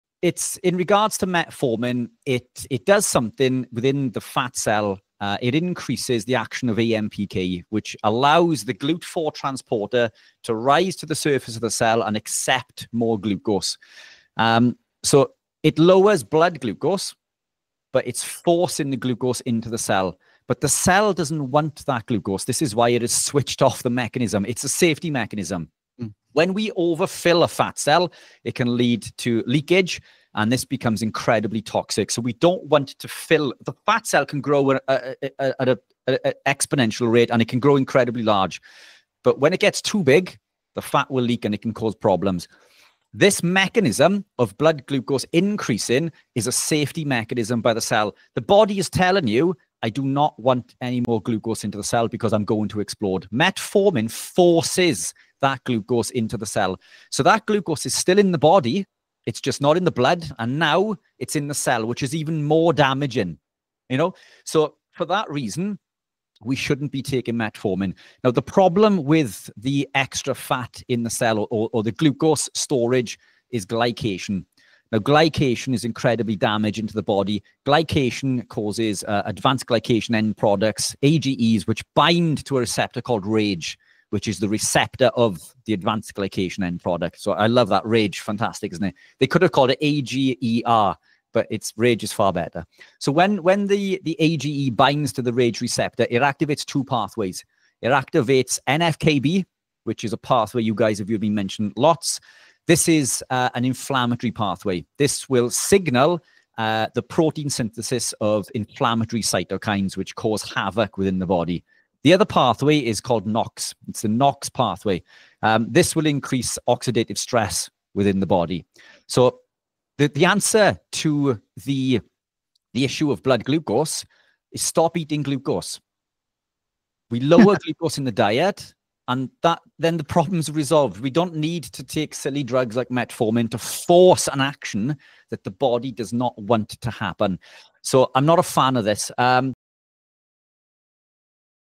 Should You Avoid Metformin When Not Diabetic?
Discover why metformin may do more harm than good if you're no longer managing type 2 diabetes. We explain how it affects fat cells, blood glucose, and the importance of dietary changes to resolve blood sugar issues naturally.